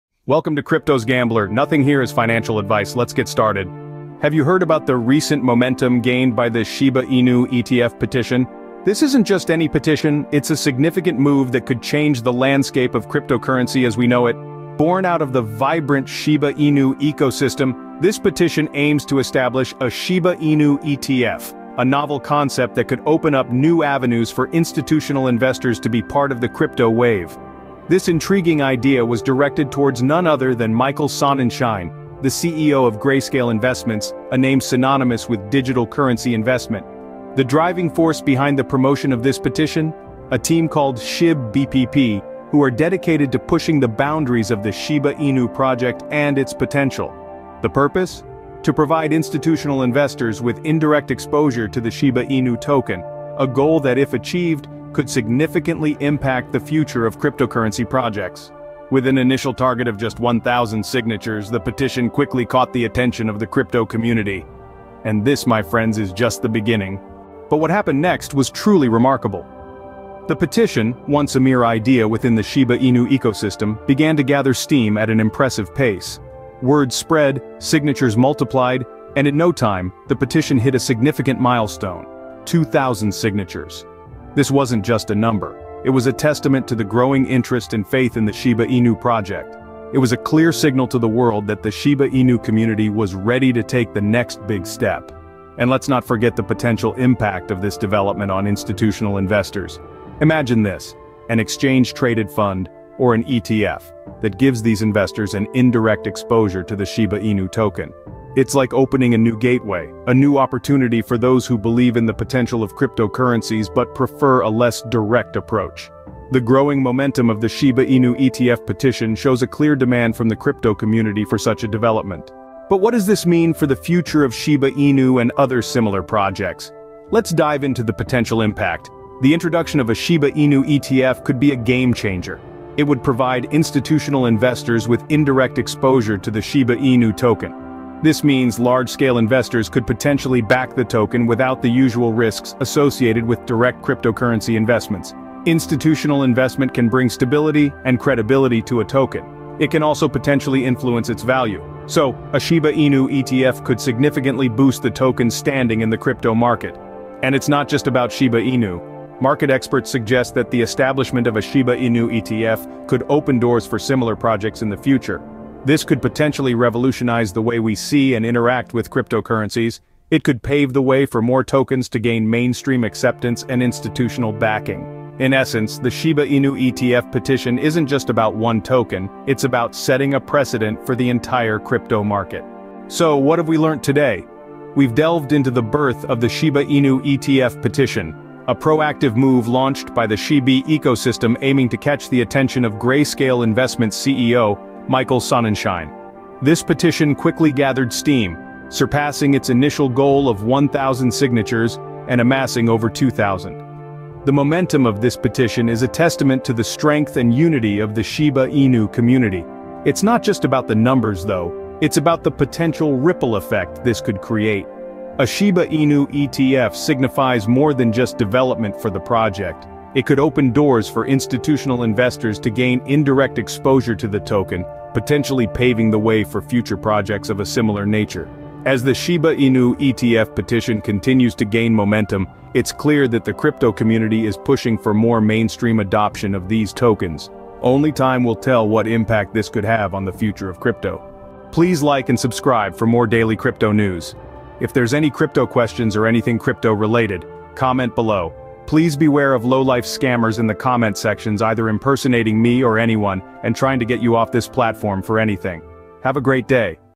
Shiba Inu Crypto ETF Petition Surges Past 2,000 Signatures in Momentum Boost
The Shiba Inu community gains traction with its petition for a Shiba Inu ETF, surpassing 2,000 signatures.